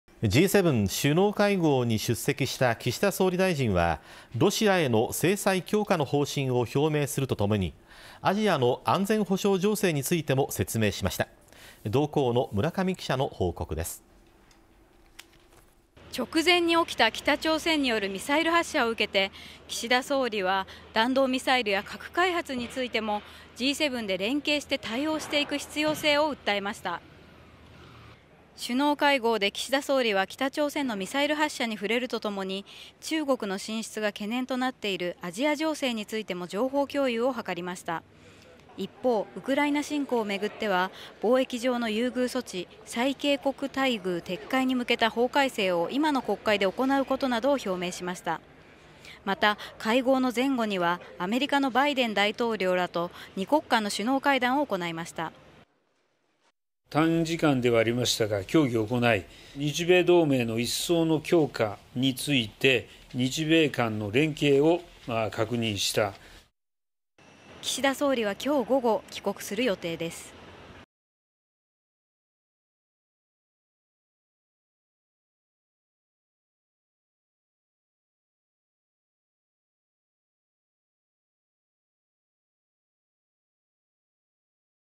岸田首相、G7出席し帰国の途 アジア情勢も情報共有 G7(主要7カ国)首脳会合
G7(主要7カ国)首脳会合に出席した岸田首相は、ロシアへの制裁強化の方針を表明するとともに、アジアの安全保障情勢についても説明した。

直前に起きた北朝鮮によるミサイル発射を受けて、岸田首相は、弾道ミサイルや核開発についても、G7で連携して対応していく必要性を訴えた。

首脳会合で岸田首相は、北朝鮮のミサイル発射に触れるとともに、中国の進出が懸念となっているアジア情勢についても情報共有を図った。

一方、ウクライナ侵攻をめぐっては、貿易上の優遇措置「最恵国待遇」撤回に向けた法改正を今の国会で行うことを表明するなどした。

また、会合の前後には、アメリカのバイデン大統領らと2国間の首脳会談を行った。

岸田首相「(バイデン大統領とは)短時間ではあったが協議を行い、日米同盟の一層の強化について、日米間の連携を確認した」

岸田首相は25日午後、帰国する予定。